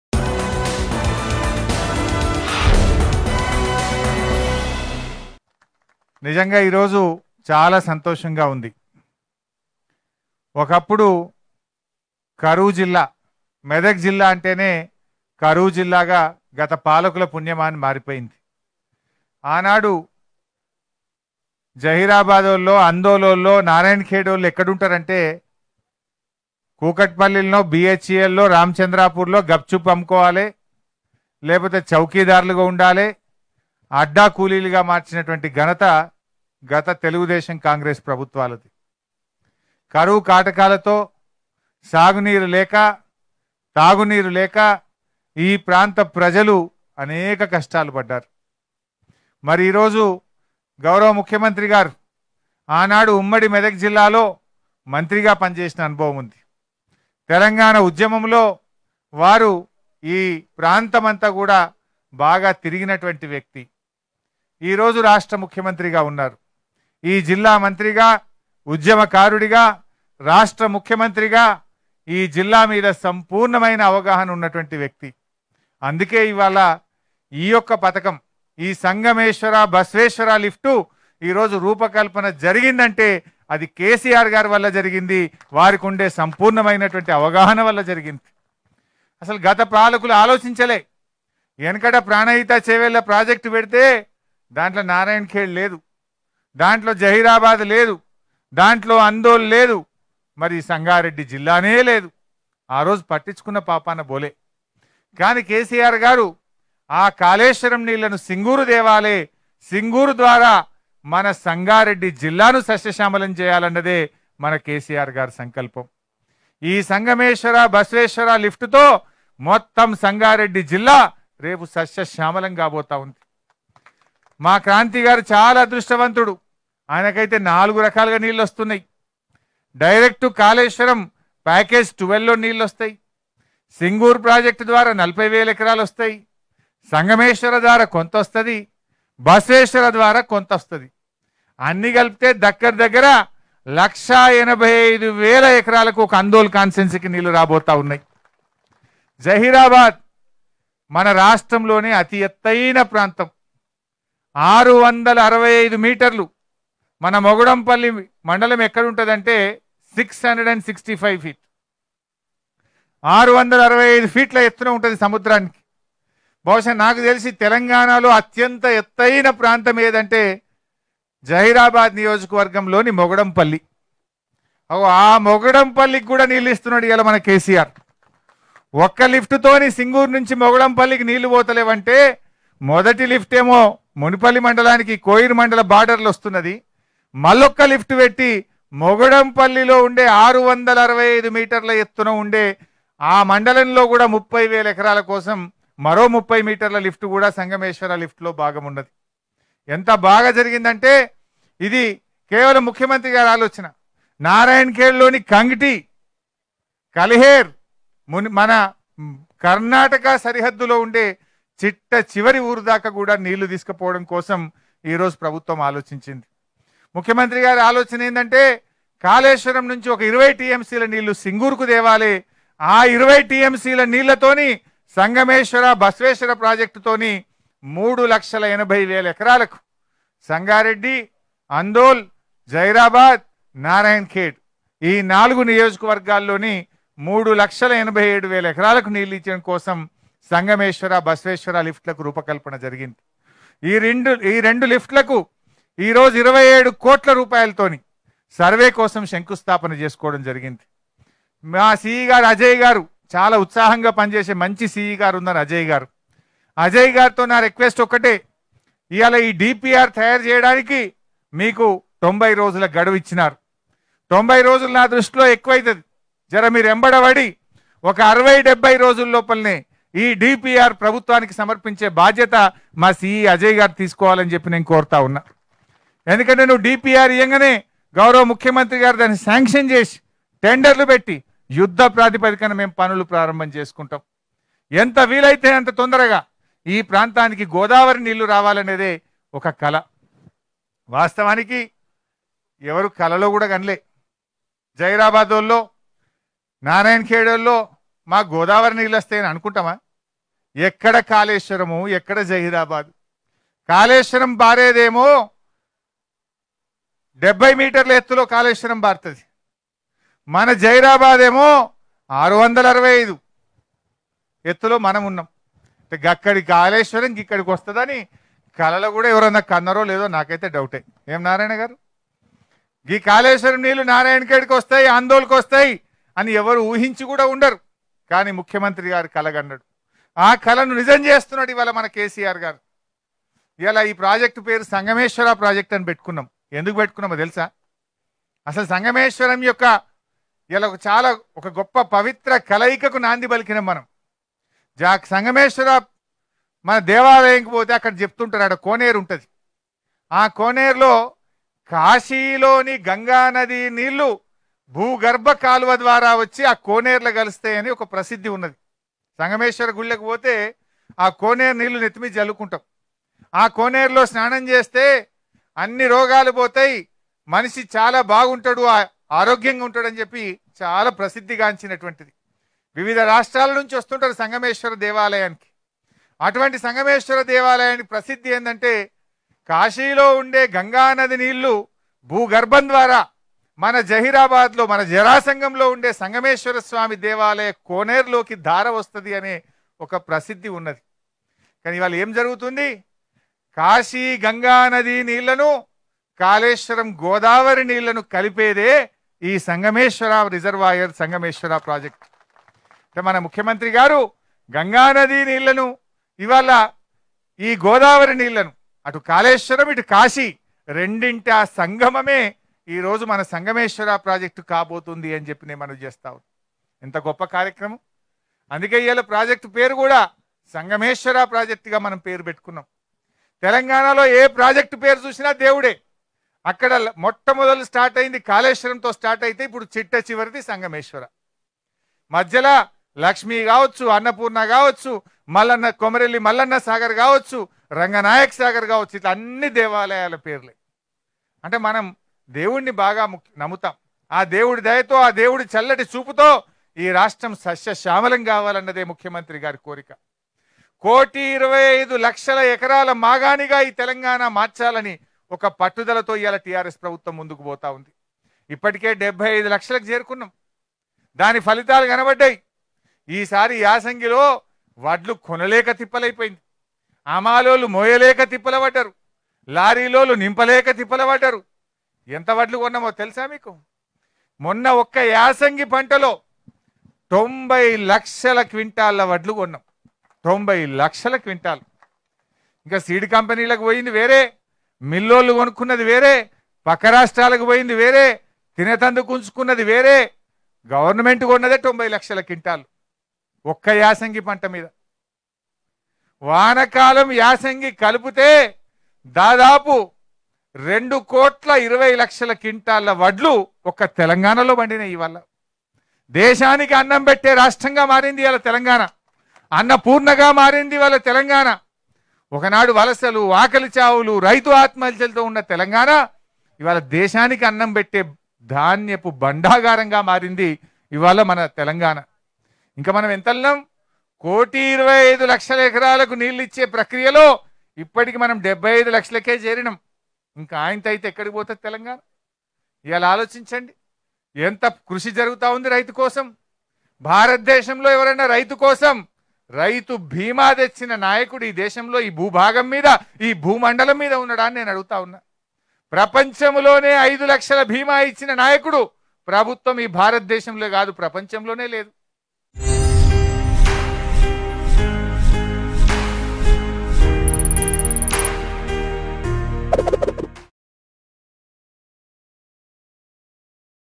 Minister Harish Rao Inaugurates Survey of Sangameshwara Lift Irrigation Project | Mango News
Minister Harish Rao Inaugurates Survey of Sangameshwara Lift Irrigation Project.